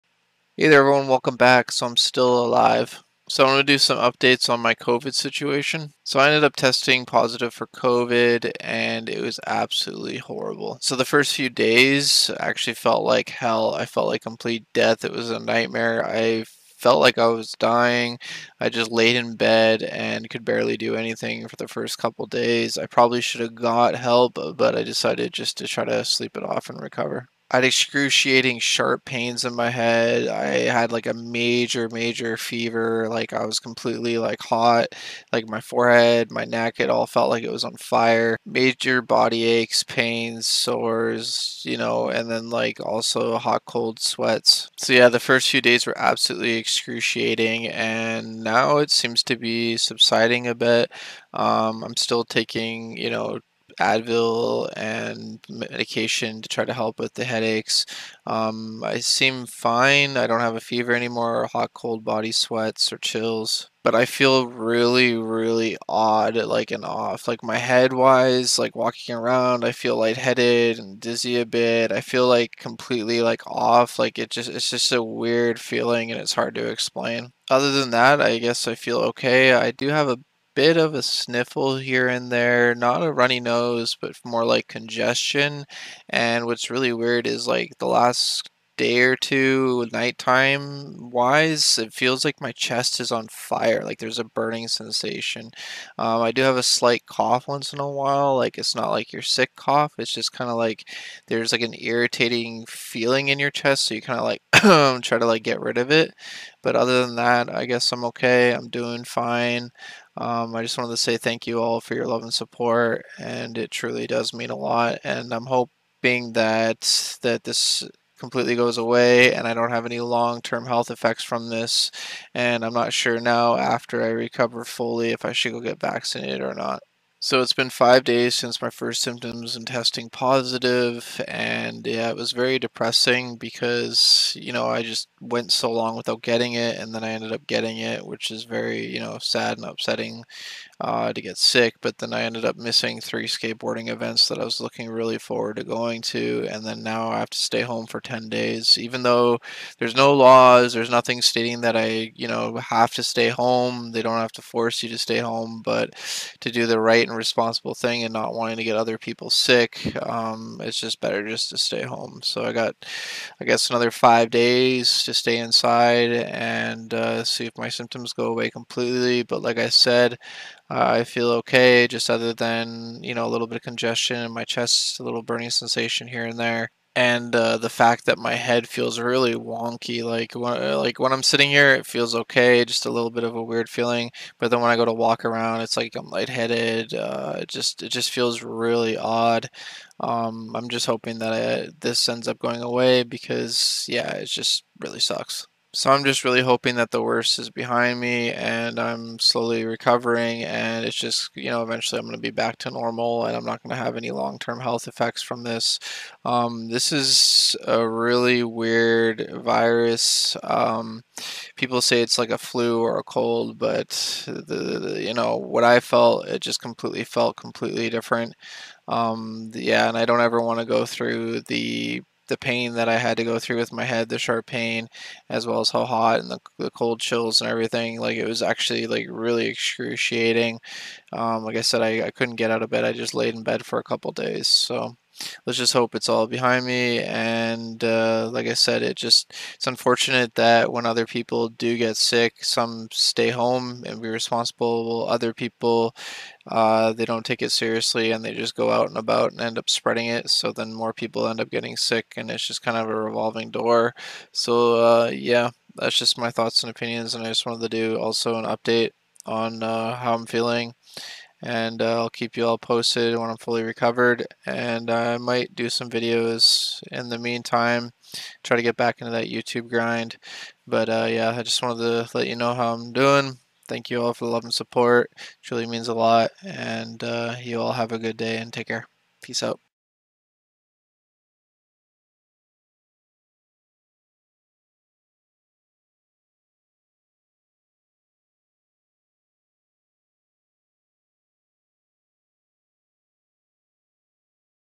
Update Since I Caught Covid-19
How I'm Doing Since I Caught Covid-19
so the first few days were like hell, the sharp pains I was getting in my head were really bad and probably should have got help..
I feel like I'm slowly getting better,
the feeling my head has is really weird and sucks, I feel light headed and dizzy and just really off...
at night time I have noticed my chest as a slight burning sensation so I'm hoping I get better and recover 100% and have no long term health problems from this..